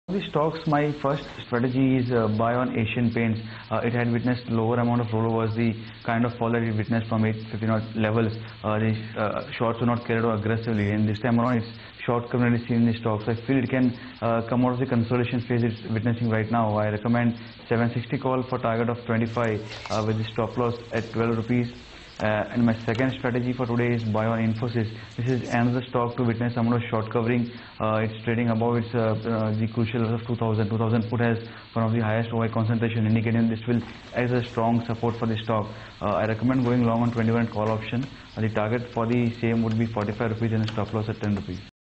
Buy Asian Paints 760 Call, Infosys 2100 Call: Gaurav Bissa
Gaurav Bissa of LKP Securities recommends buying Asian Paints 760 Call and advises going long in Infosys 2100 Call.